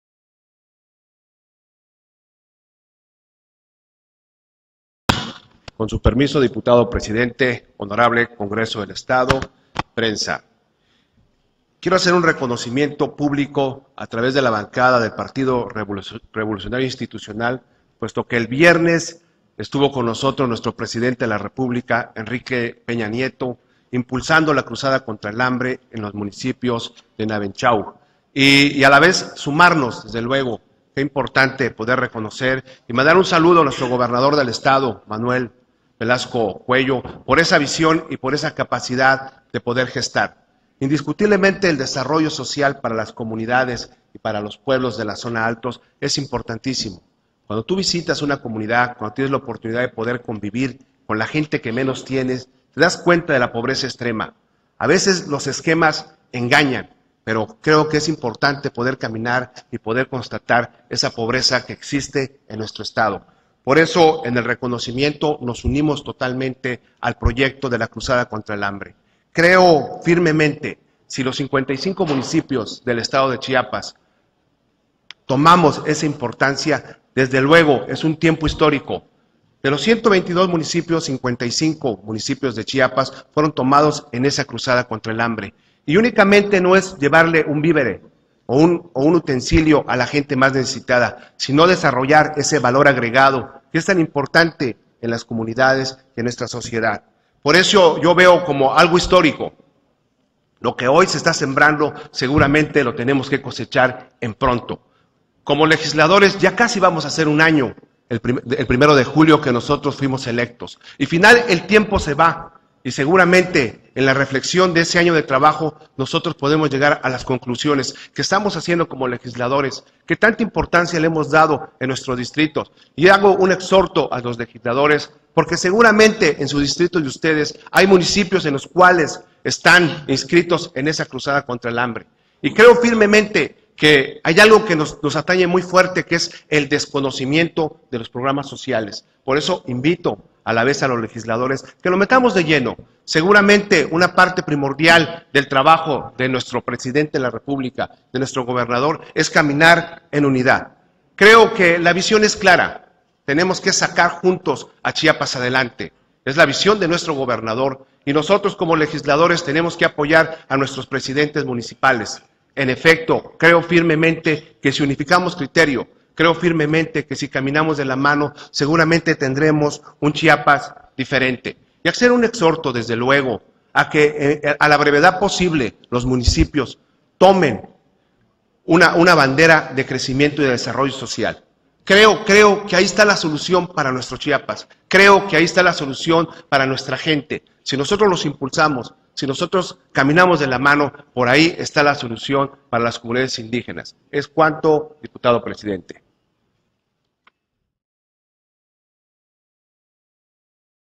Intervención Dip. Marco Antonio Cancino González Sesión Ordinaria 23 de Abril de 2013
Sesión Ordinaria del día 23 de Abril de 2013, correspondiente al Segundo Periodo Ordinario de Sesiones del Primer Año de Ejercicio Constitucional, celebrada por la LXV Legislatura del H. Congreso del Estado Libre y Soberano de Chiapas.

Lugar: Pleno
Fecha: 23 de Abril de 2013
Elaborado: Unidad de Informática